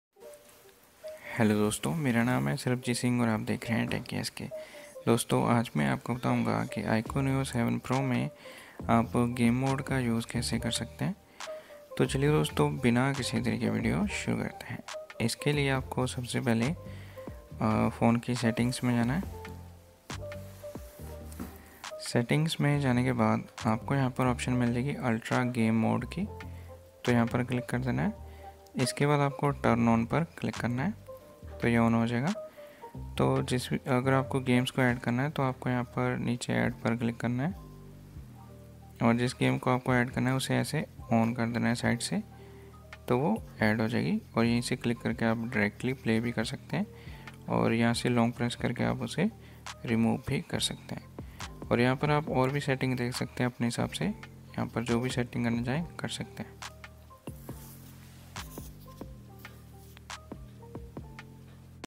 How to use Game Space on Iqoo Neo 7 Pro 5G || Iqoo Neo 7 Pro 5G me game space kaise use kare
Hello dosto, aaj maine is video me bataya hai ki Iqoo Neo 7 Pro 5G me game space kaise use kare, dosto Iqoo Neo 7 Pro 5G me game space use karna bahut hi aasan hai. Agar aap bhi janna chahate hain ki Iqoo Neo 7 Pro 5G me game space kaise use kare to is video ko pura dekhiyega, thanks friends.

Hello friends, in this video, I've explained how to use Game Space on Iqoo Neo 7 Pro 5G. Using Game Space on Iqoo Neo 7 Pro 5G is very easy. If you want to know how to use Game Space on Iqoo Neo 7 Pro 5G, be sure to watch this entire video. Thanks, friends.